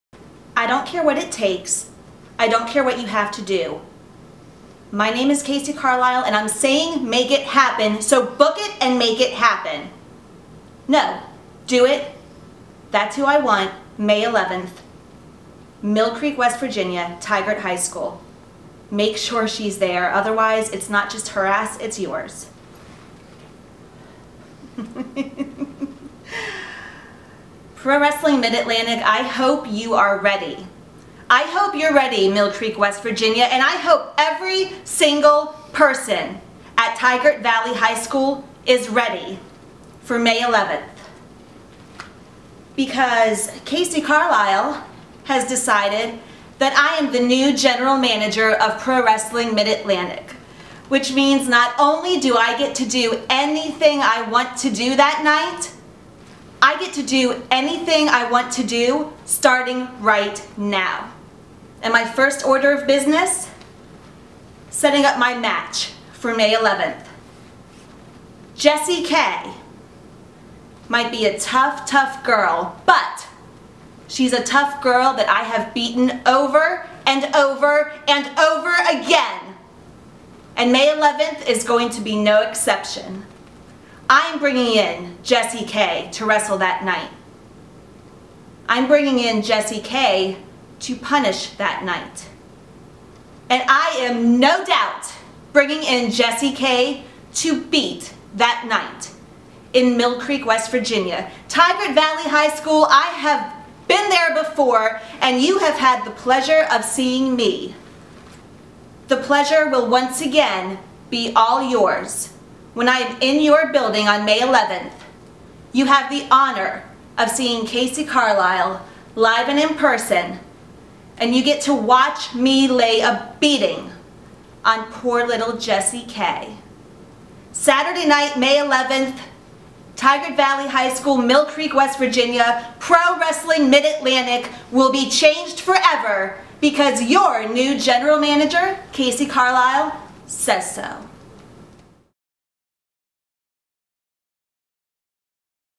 Kacee Carlisle with a HUGE Pro Wrestling Mid-Atlantic Update!!!!
Execs at Pro Wrestling Mid-Atlantic had their clocks ROCKED today after hearing this trailblazing message from Kacee Carlisle. This is the FUTURE. Keith Krockett Promotions and Pro Wrestling Mid-Atlantic PROUDLY bring to you May 11th, 2013 Mill Creek, West Virginia at Tygart Valley High School 7:30 bell time, get ready for MILL CREEK MAYHEM 2013. THIS WILL BE A T.V. TAPING!